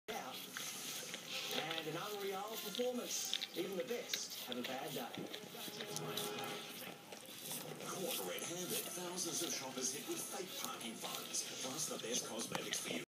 Diesel, with lots of tongue
Diesel dog licking my plate clean after I ate some pizza in front of the TV